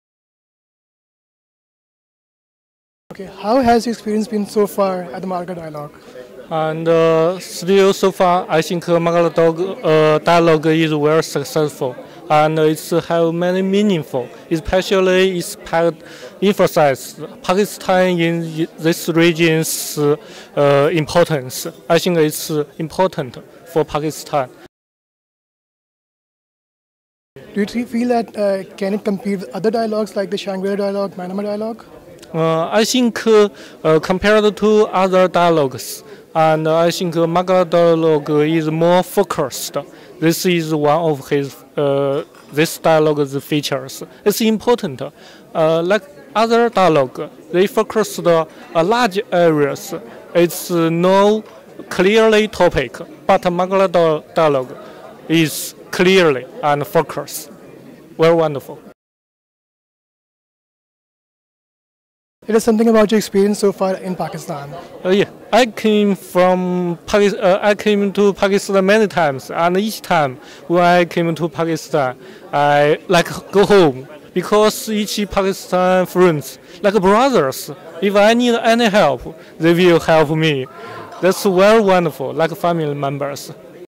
Dr Li JingFeng - Margalla Dialogue'19
Dr Li JingFeng, Director, Regional Studies and Strategies Research Center, Sichuan Academy of Social Sciences (SASS), China sharing his thoughts at the MargallaDialogue2019.